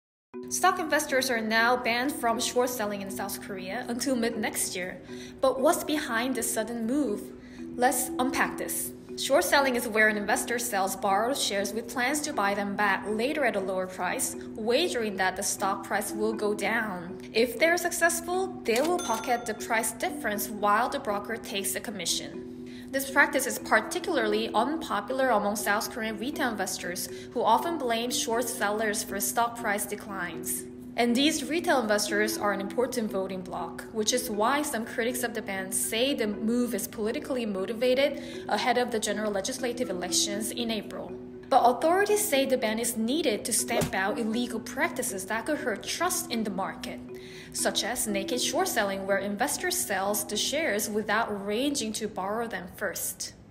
Why South Korea Is At War With Short Sellers, Again
What is short selling and why has it been banned from South Korea's $1.7 trillion equity market?

Youkyung Lee explains what is behind this controversial move.